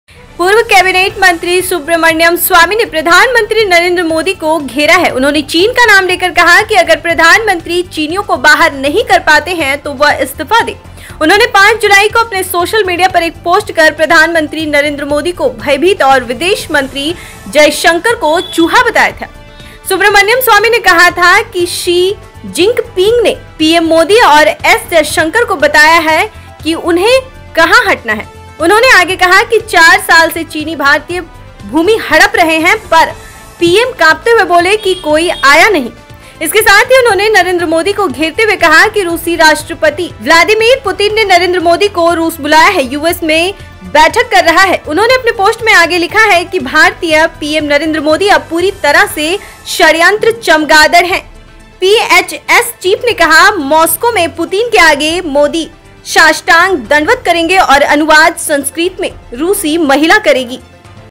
Subramanian Swamy: पूर्व कैबिनेट मंत्री SubramanianSwamy ने PM Narendra Modi को घेरा, दिया बड़ा बयान
पूर्व कैबिनेट मंत्री सुब्रमण्यम स्वामी ने प्रधानमंत्री नरेंद्र मोदी को घेरा है। उन्होंने चीन का नाम लेकर कहा कि अगर प्रधानमंत्री चीनियों को बाहर नहीं कर पाते हैं तो वह इस्तीफा दे। उन्होंने 5 जुलाई को अपने सोशल मीडिया एक पर पोस्ट कर प्रधानमंत्री नरेंद्र मोदी को भयभीत और विदेश मंत्री जयशंकर को चूहा बताया था